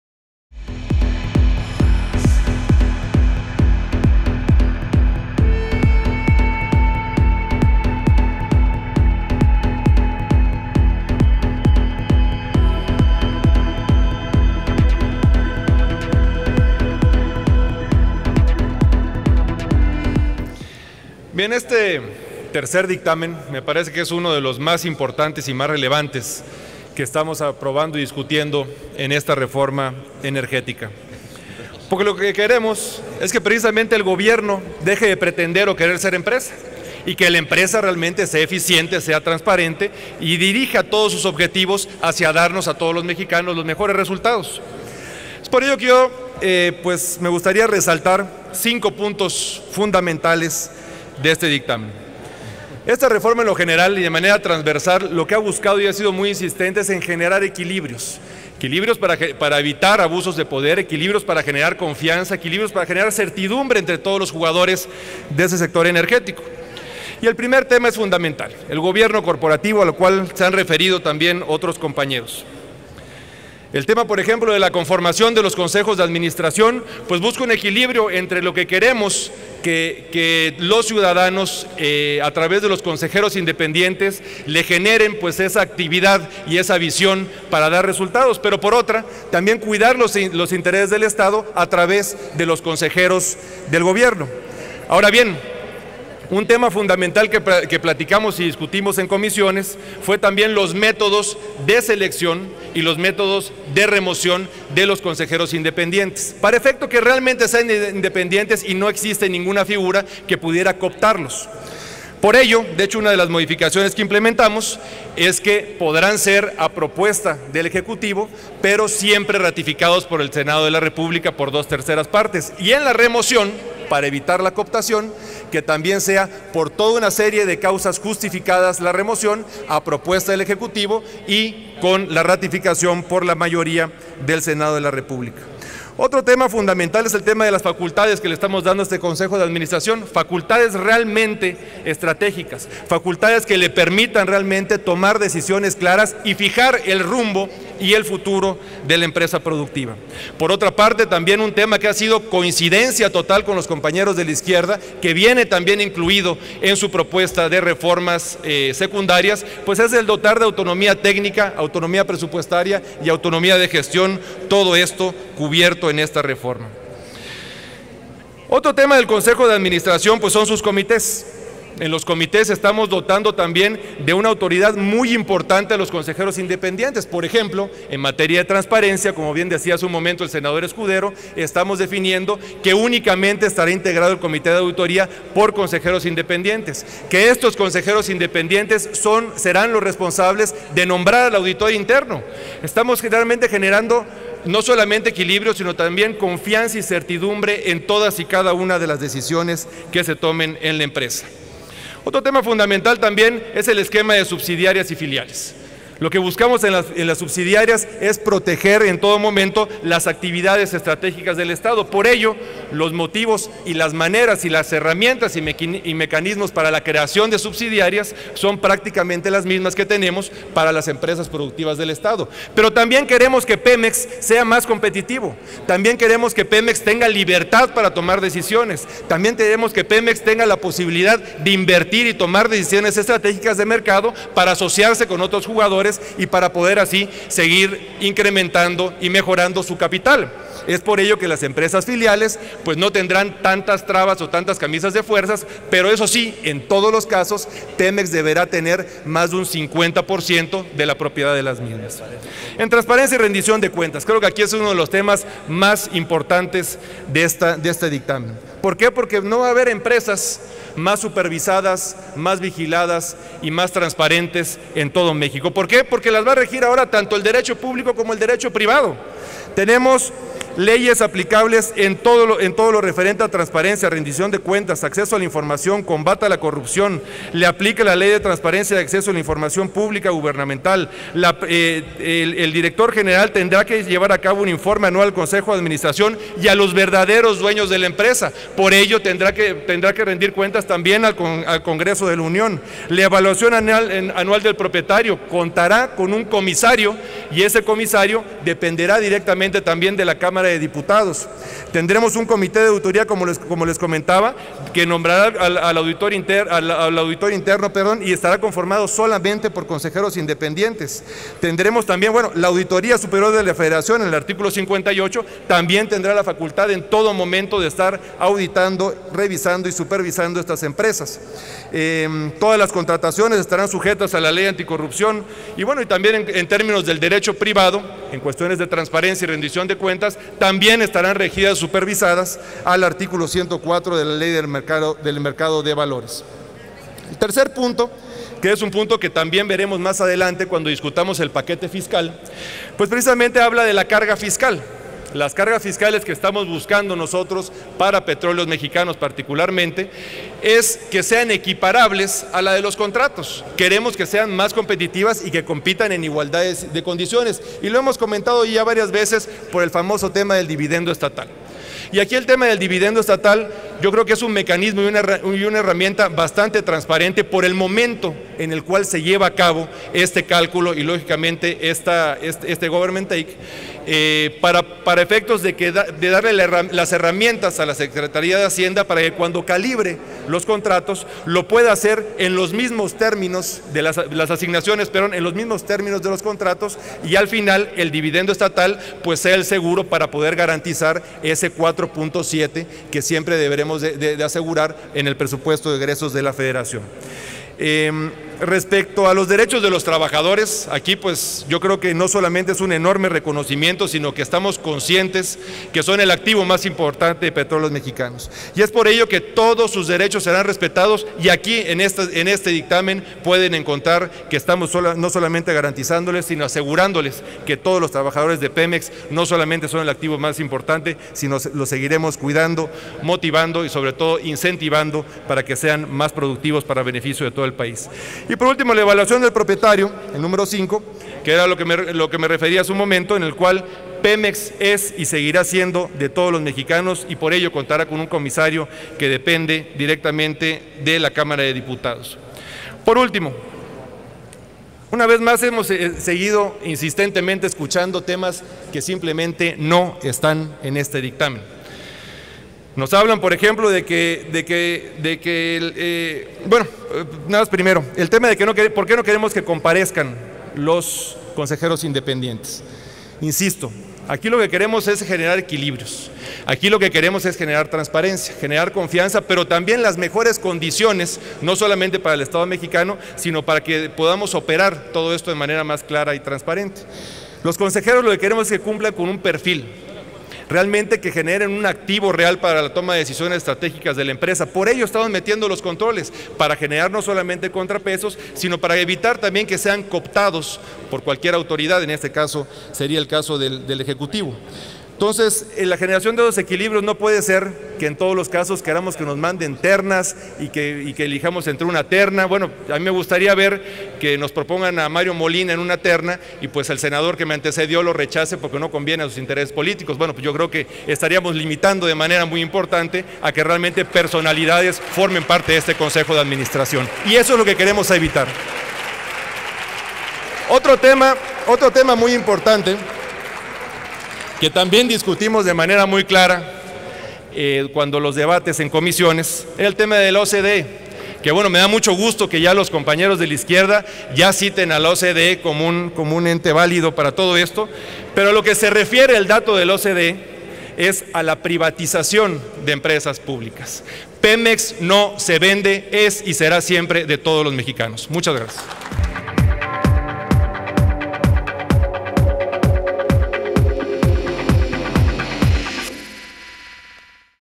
Posicionamiento del GPPAN sobre tercer dictamen de Reforma Energética - Sen. Lavalle - 200714
México, D.F., 20 de julio de 2014.- Intervención en tribuna del senador Jorge Luis Lavalle Maury al emitir el posicionamiento del GPPAN sobre el dictamen que contiene proyecto de decreto por el que se expiden la Ley de Petróleos Mexicanos y la Ley de la Comisión Federal de Electricidad, y se reforman y derogan diversas disposiciones de la Ley Federal de las Entidades Paraestatales; la Ley de Adquisiciones, Arrendamientos y Servicios del Sector Público y la Ley de Obras Públicas y Servicios Relacionados con las Mismas.